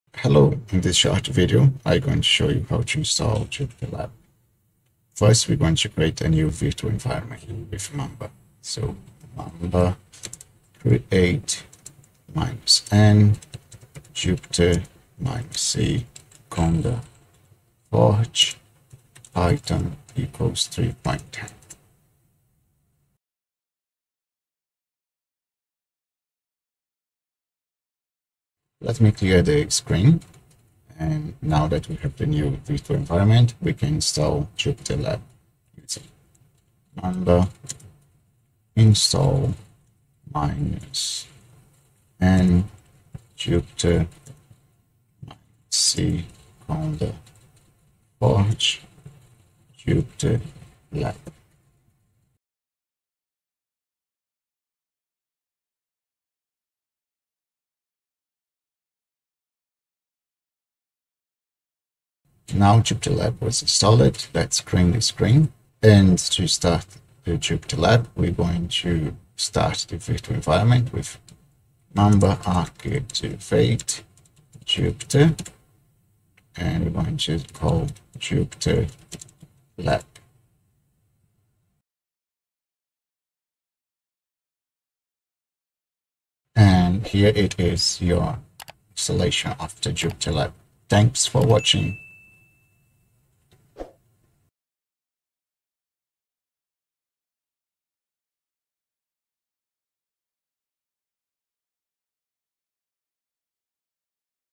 How to install JupyterLab using Mamba
In this short tutorial, I show you how to install JupyterLab using the cross-platform package manager Mamba.

Shell Instructions

$ mamba create -n jupyter -c conda-forge python=3.10
$ mamba install -n jupyter -c conda-forge jupyterlab
$ mamba activate jupyter
$ jupyter lab

Session Information

OS (GNU/Linux distribution): Ubuntu 21.10
Windowing System: X
Mamba 0.21.1

Screencast and Video Editing Information

VirtualBox 6.1
OBS Studio 27.0
Kdenlive 21.04